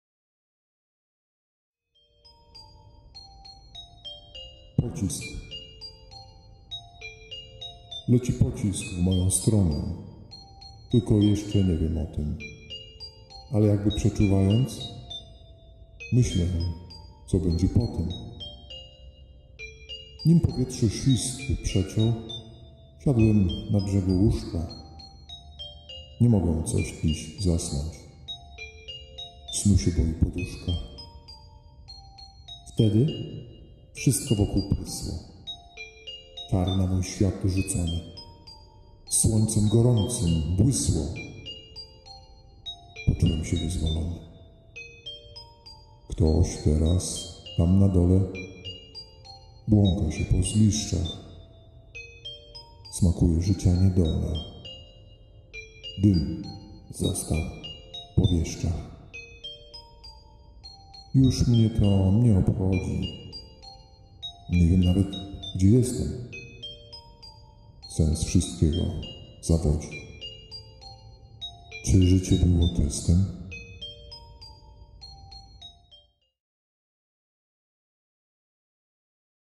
POCISK
Leci pocisk w moją stronę
Tylko jeszcze nie wiem o tym
Ale jakby przeczuwając
Myślę co będzie po tym.

Nim powietrze świst przeciął
Siadłem na brzegu łóżka
Nie mogę coś dziś zasnąć
Snu się boi poduszka.

Wtedy wszystko wokół prysło
Czar na mój świat rzucony
Słońcem gorącym błysło
Poczułem się wyzwolony.

Ktoś teraz, tam na dole,
Błąka się po ruinach
Smakuje życia niedolę.
I ból straty po synach.

Już mnie to nie obchodzi
Nie wiem nawet gdzie jestem.
Sens wszystkiego zawodzi.
Czy życie było testem?

copyright @rymonauta